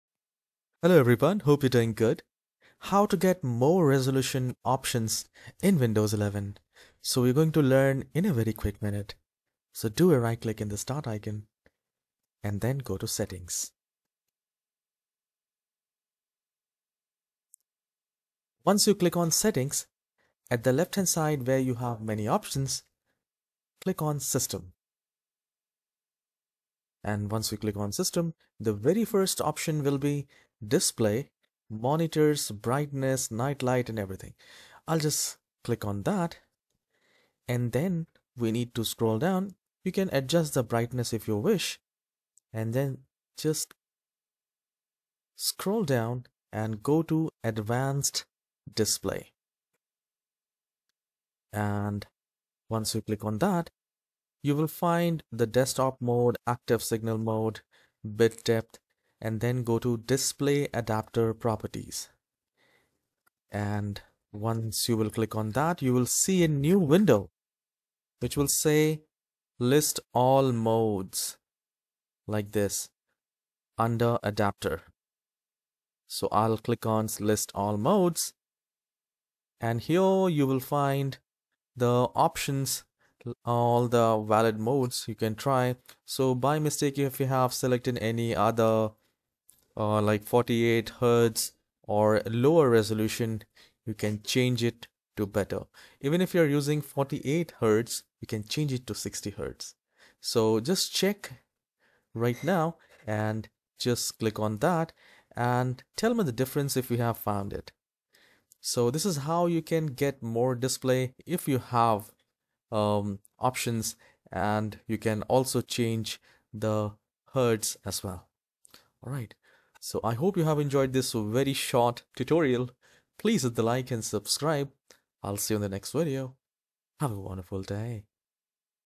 How to Get More Resolution Options in Windows 11 [2024 Update]
Are you struggling with limited resolution options on your Windows 11 system? Look no further! In this comprehensive guide, we'll show you exactly how to expand your resolution choices and unlock the full potential of your display settings.

Whether you're a gamer, designer, or simply someone who values crisp visuals, having access to a wide range of resolution options is crucial. With the latest updates to Windows 11, there are several methods you can use to enhance your viewing experience.

In this video, we'll walk you through step-by-step instructions on how to tweak your display settings to access additional resolution options. From adjusting scaling settings to updating your graphics drivers, we'll cover everything you need to know to get the most out of your display.

Additionally, we'll explore advanced techniques for optimizing resolution options, including utilizing third-party software and customizing advanced display settings. By the end of this video, you'll have all the tools and knowledge you need to tailor your resolution settings to suit your preferences perfectly.

Don't let limited resolution options hold you back any longer. Watch this video now and take control of your Windows 11 display settings like a pro!